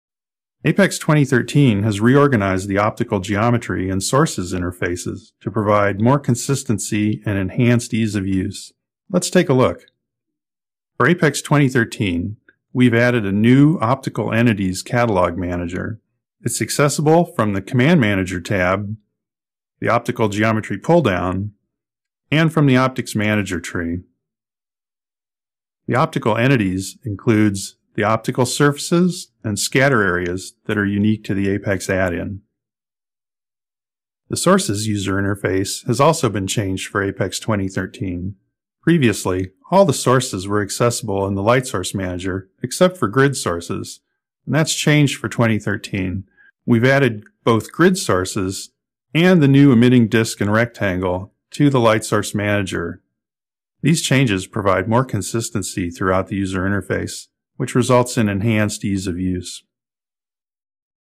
Optical Entities Catalog Manager in APEX
APEX now has updated optical geometry and sources interfaces for greater consistency and ease of use. A new Optical Entities catalog manager is accessible from the Command Manager tab, the Optical Geometry pull-down menu, and the Optics Manager tree. Optical entities include optical surfaces and scatter areas unique to the APEX add-in. In addition, the Sources tab now provides easy access to all source types.